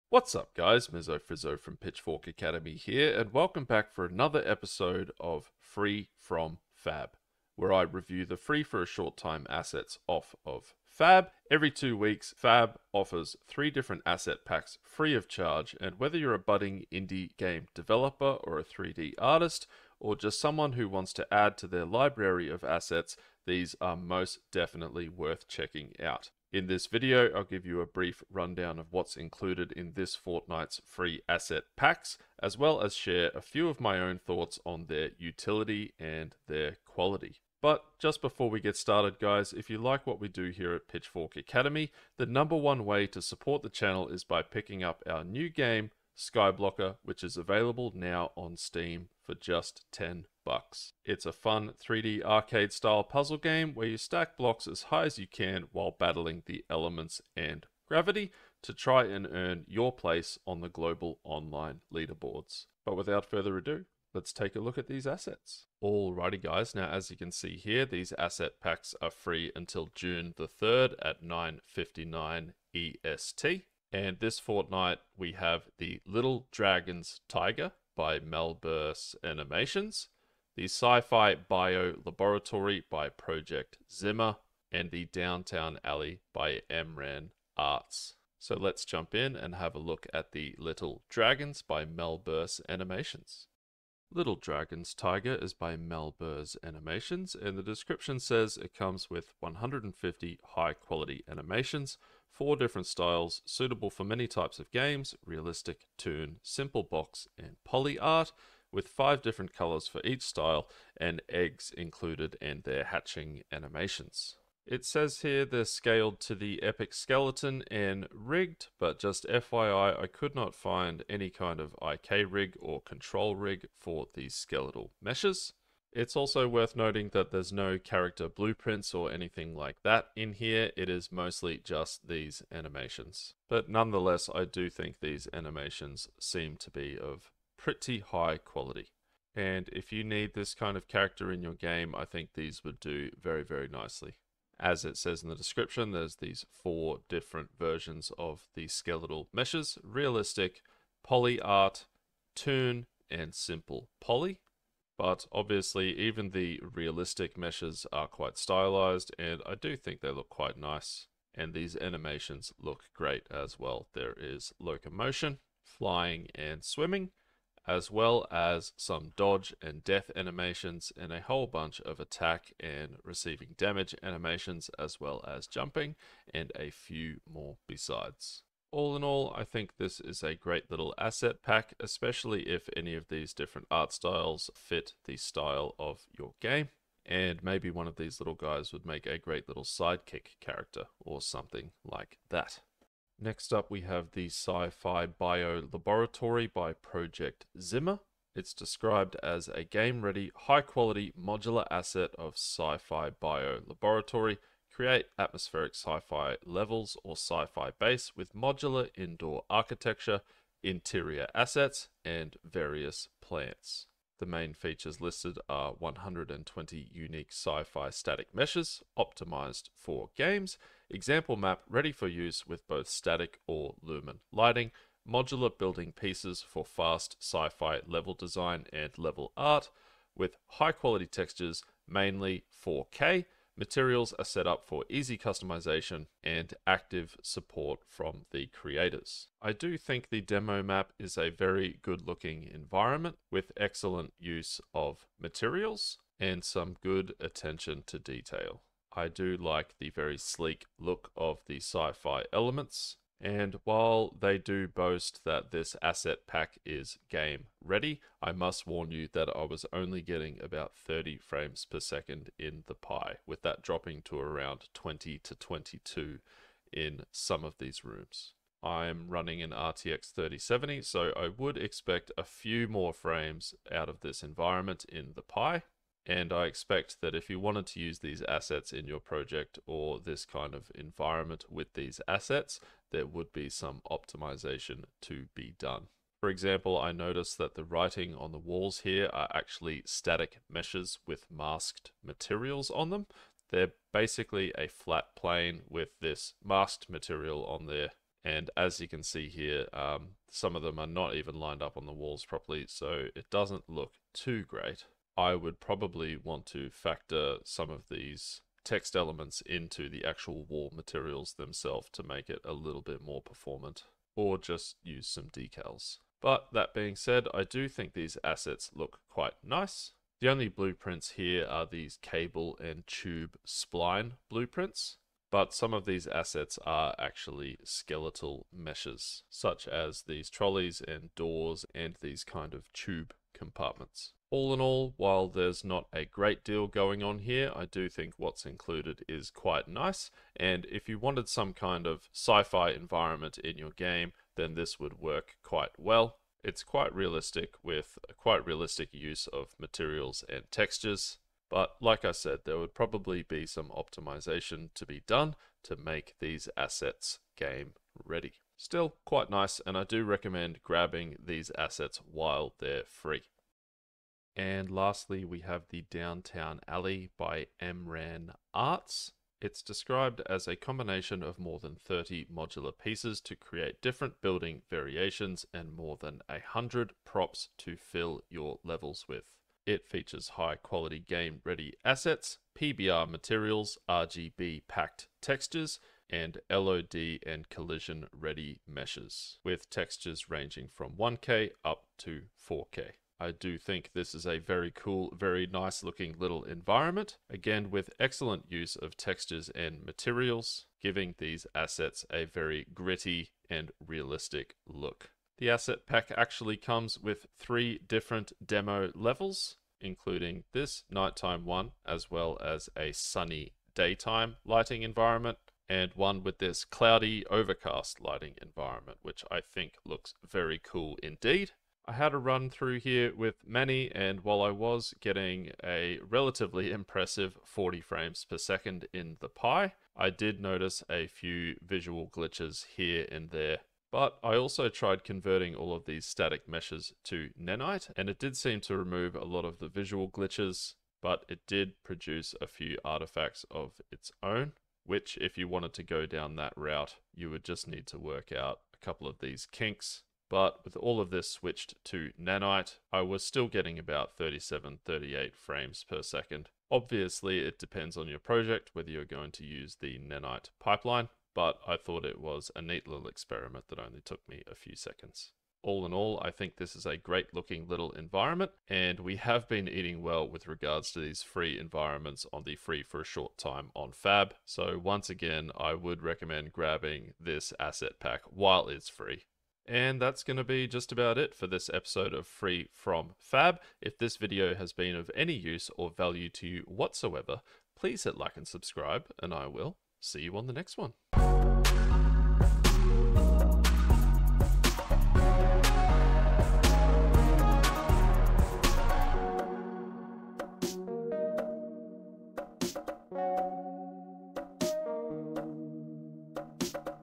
Limited-Time Free Fab Marketplace Assets (May 20th) - Unreal Engine 5 Content Review
Grab these Fab assets while they're still free! Offer expires June 3, 2025, at 9:59am ET)

In this Unreal Engine 5 video, MizzoFrizzo will showcase this fortnight's free Fab marketplace assets, as well as give you a few thoughts on the quality of the asset packs and what's included.

Pitchfork has released their first game, Skyblocker! You can support what we do here at Pitchfork Academy by picking it up on Steam today!

Timestamps:

0:00 - Intro
1:05 - What's free on Fab right now?
1:34 - Little Dragons Tiger by MalberS Animations
3:13 - Sci-Fi Bio Laboratory by Project Zima
6:04 - Downtown Alley by Emran Arts
7:59 - Outro